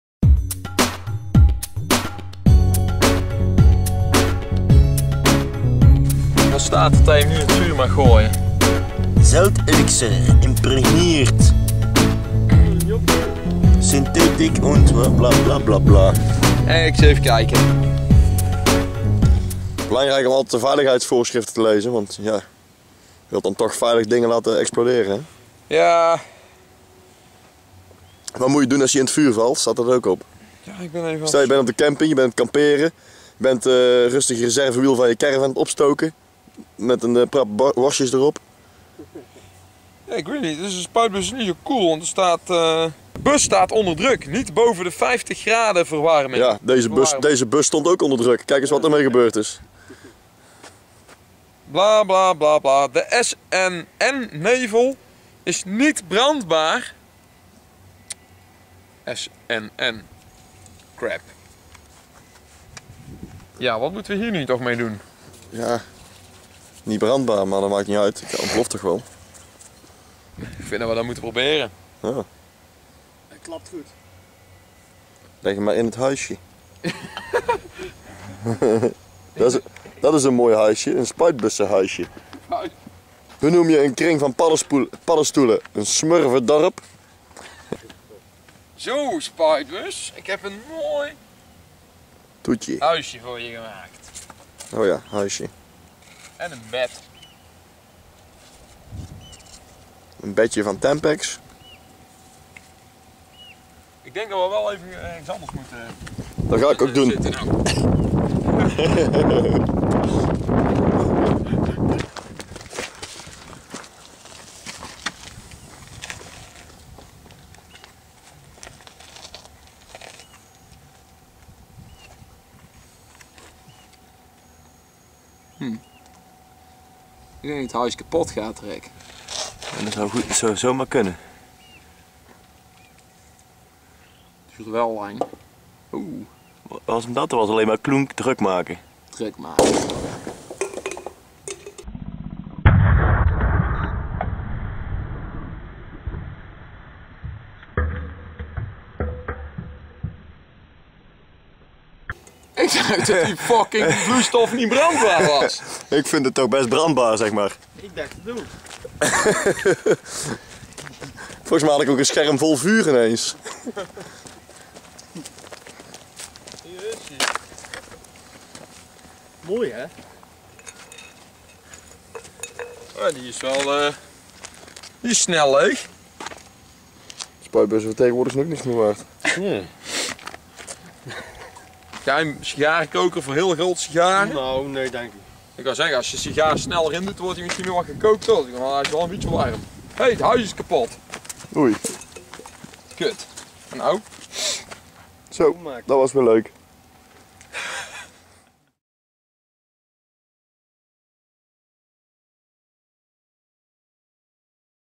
Spraycan explosion
Not flammable doesn't mean not explosive.

Niet brandbaar wil niet zeggen niet explosief.